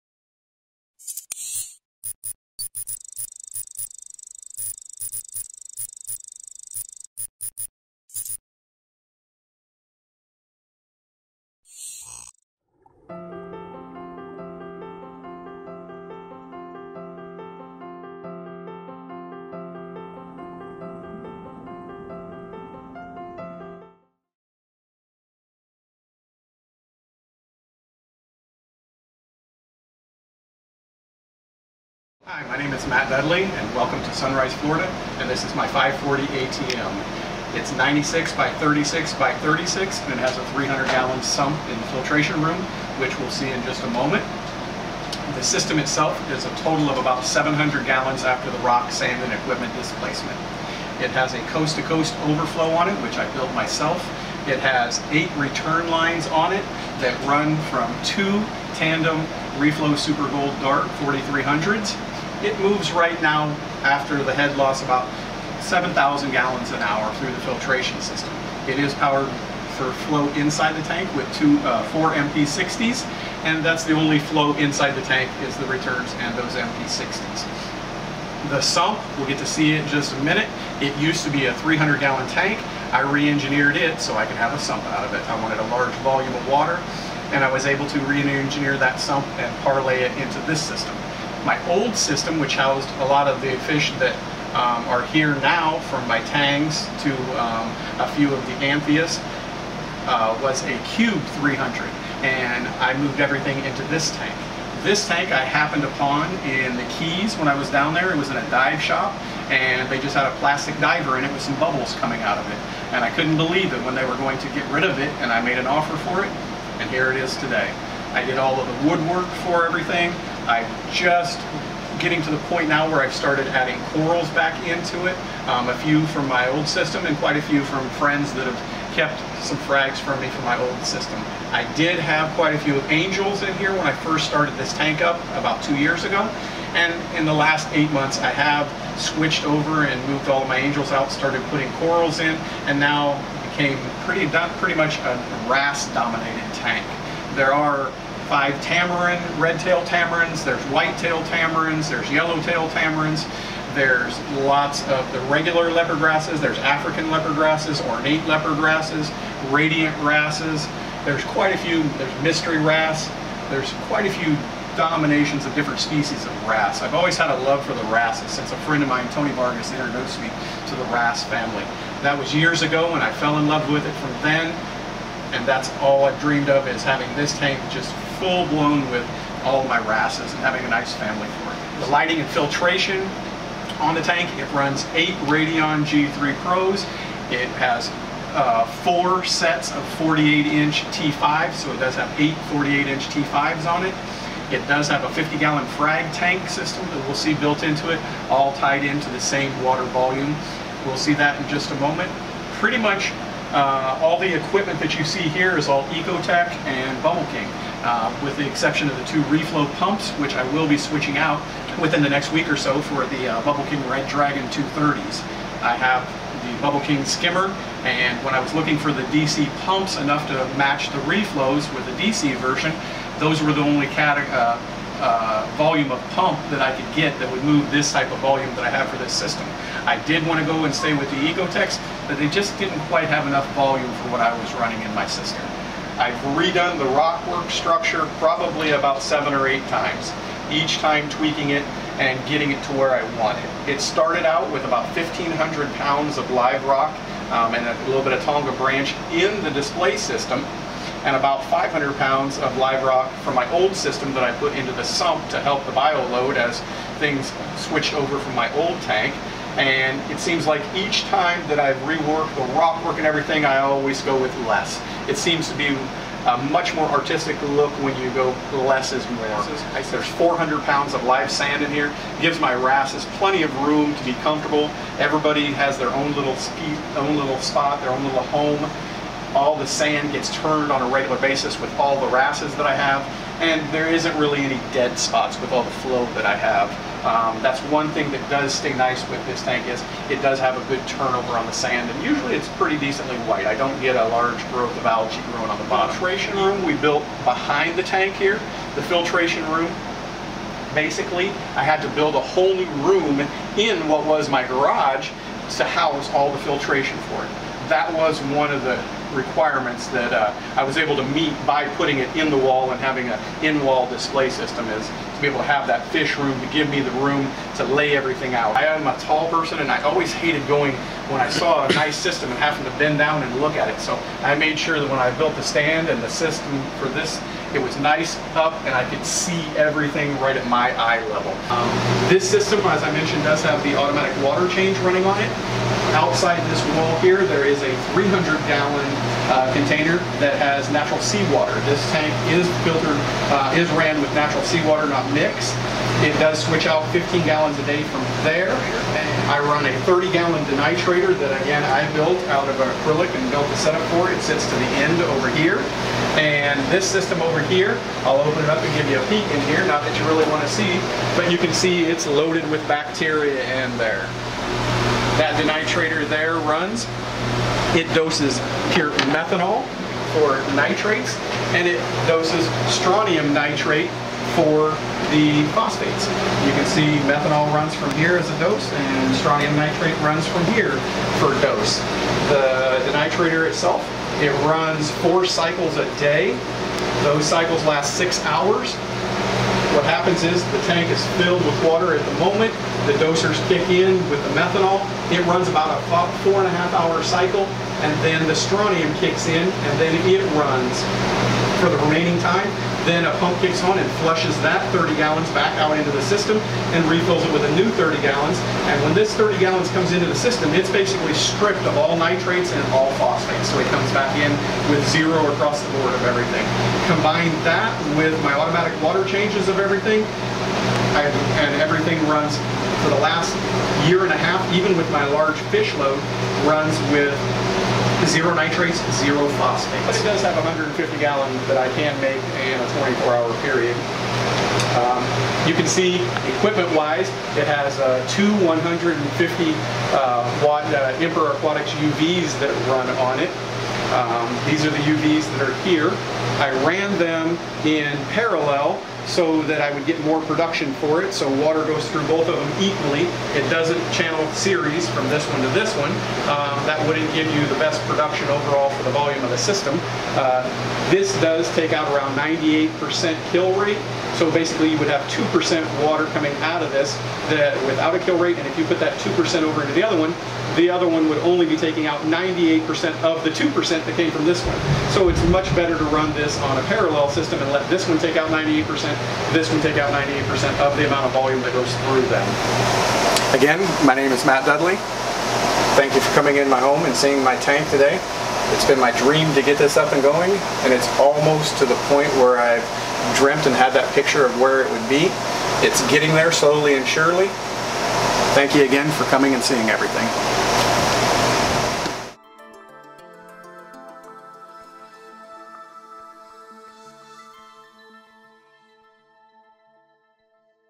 Reef2Reef TV Episode 4
In this episode, we visit Sunrise, Florida to check out Matt Dudley's beautiful ATM tank!

Top notch equipments and really well put together with good engineering. You guys will love it!